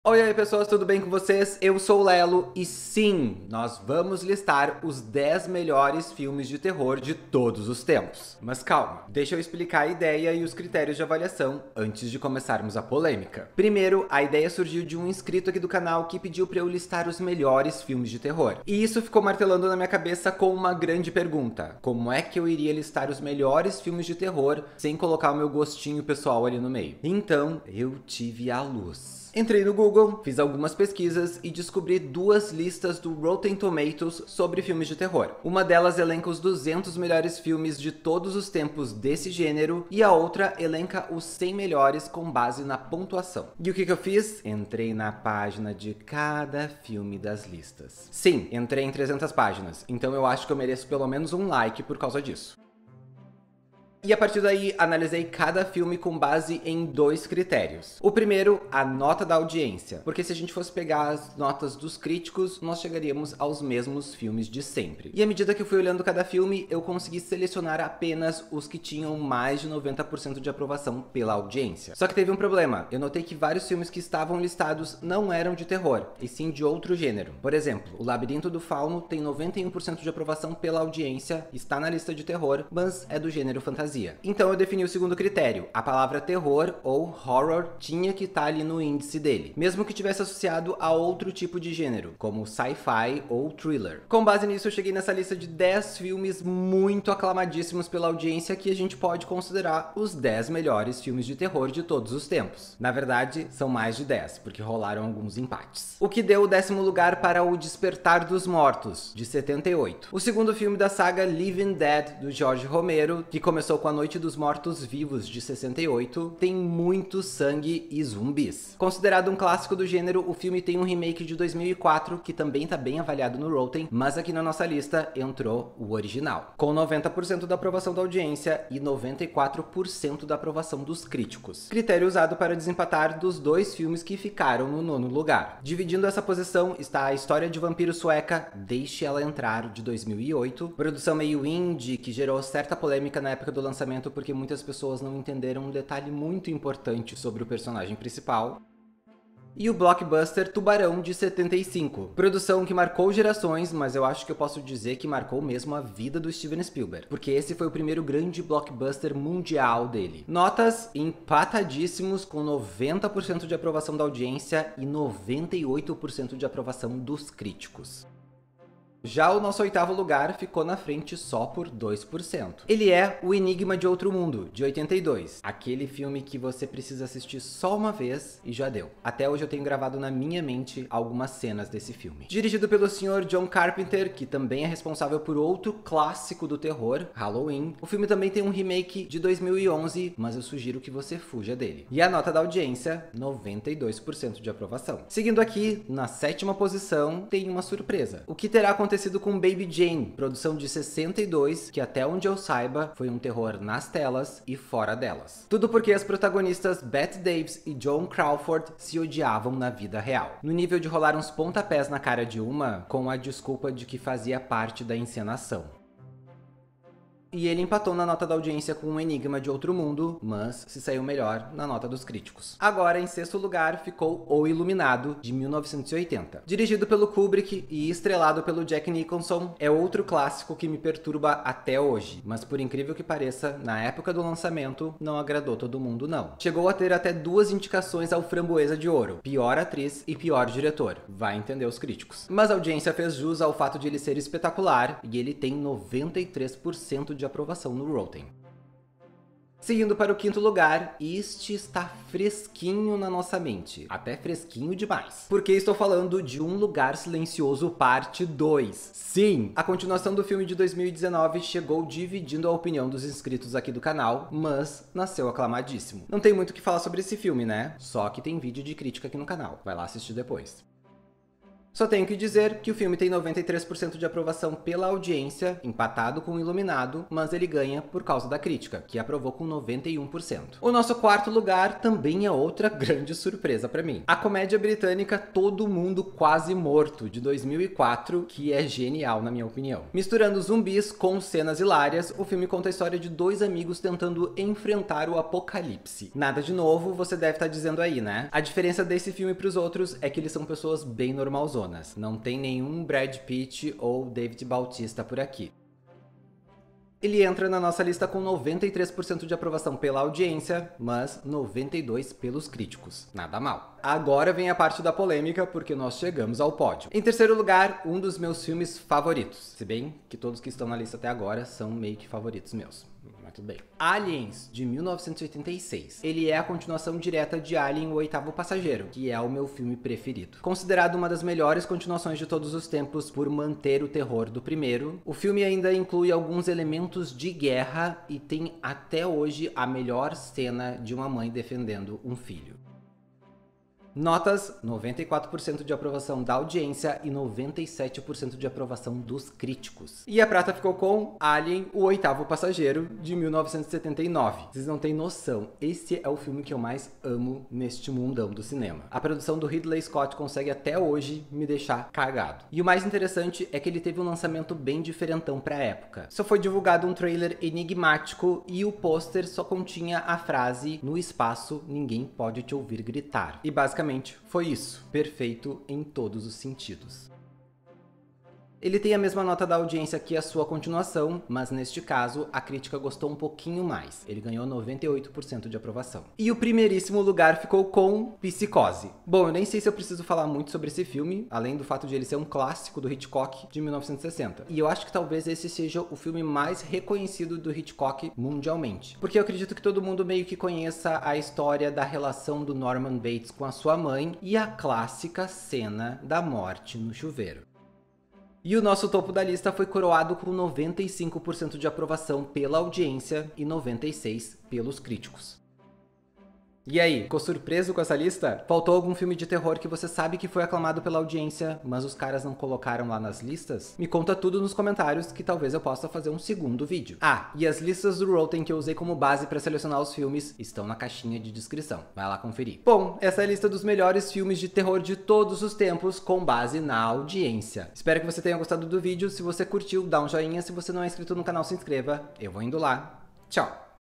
OS 10 MELHORES FILMES DE TERROR DE TODOS OS TEMPOS! Escolhidos pela audiência…
OiOi pessoas! Um inscrito aqui do canal me pediu para listar os melhores filmes de terror. Para não apenas a minha opinião, resolvi fazer algo diferente. Saí a procura de algumas listas e encontrei duas do Rotten Tomatoes! Uma com os 200 melhores filmes de terror de todos os tempos e uma com os melhores filmes de terror com base na pontuação. O que fiz? Criei dois critérios e consegui chegar aos 10! E tiveram várias surpresas!

Você concorda com a lista? Faltou algum que se encaixa nos critérios?

Deixa nos comentários que posso fazer uma lista nova!